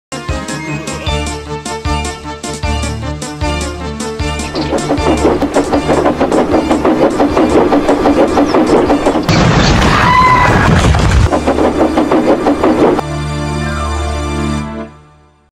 Jeremy And The snow |Lego Trains
Yes there is blue tack holding the funnel in place I don’t have the right piece. Sorry I haven’t uploaded a real video in ages but here u go new video. Let me know if you want more of ‘Theodore’ in the future.